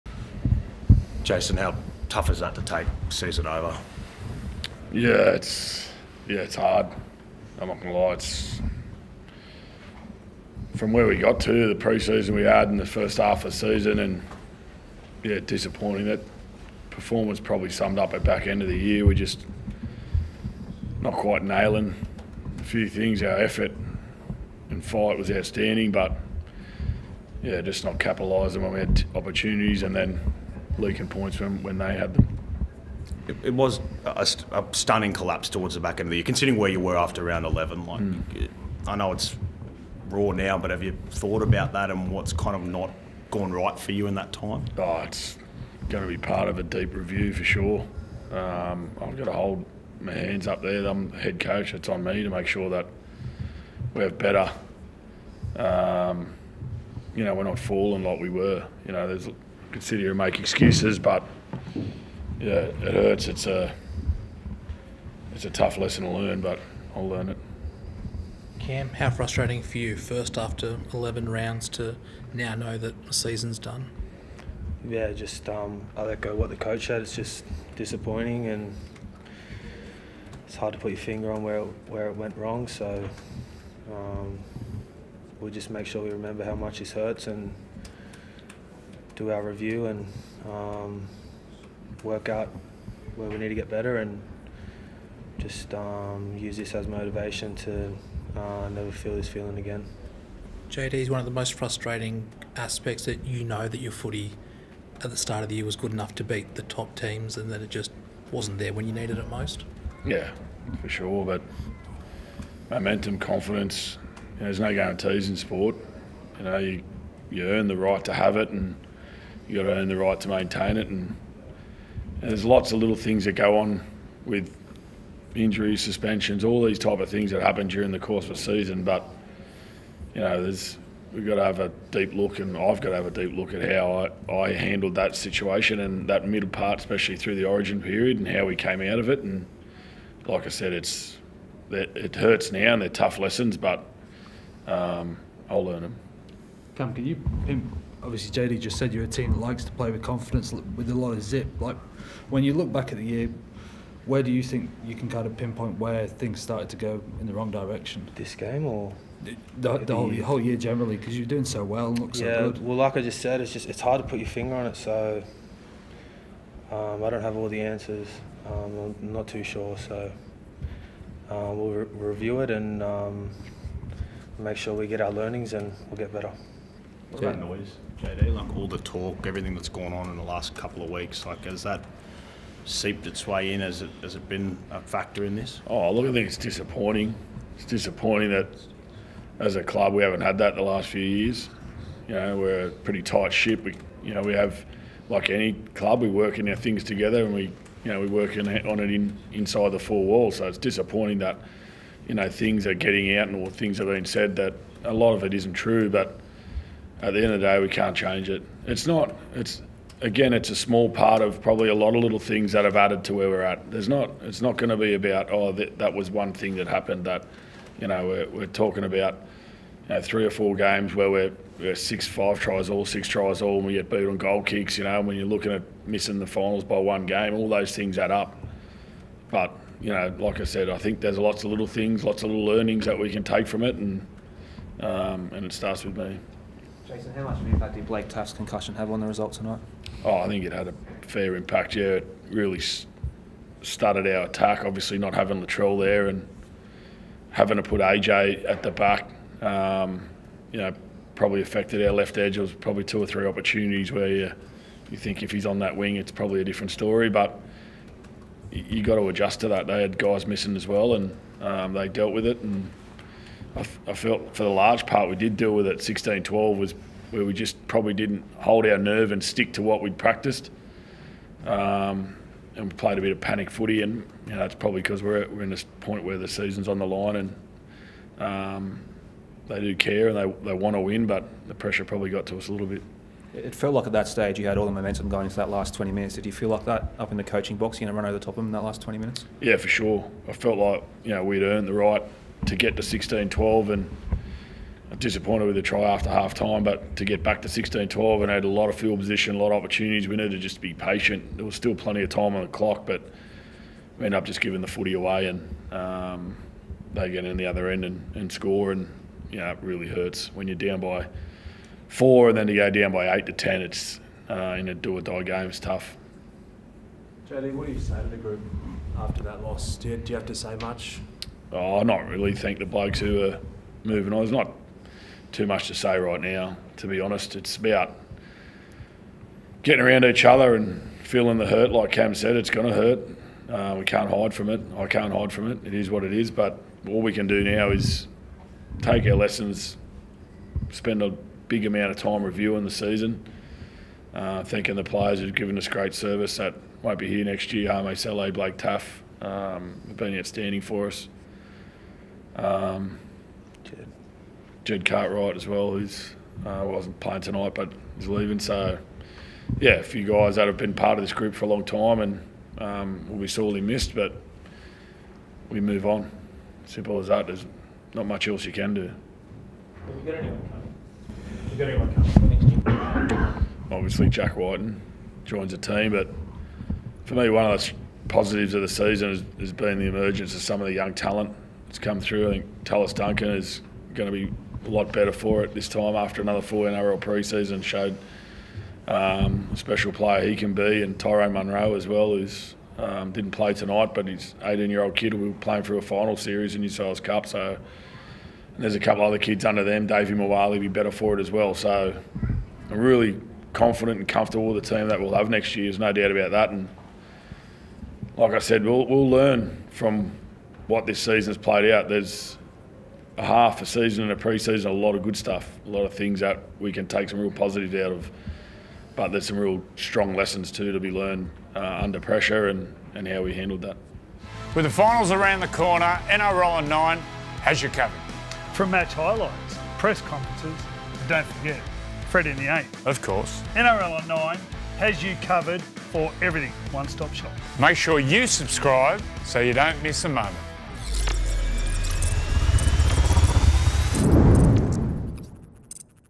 Demetriou speaks out after the Rabbitohs crash out of the top 8: NRL Presser | NRL on Nine
South Sydney Rabbitohs coach Jason Demetriou and captain Cameron Murray have addressed the media after their side crashed out of the top 8 after losing to the Sydney Roosters in round 27 of the 2023 NRL Premiership.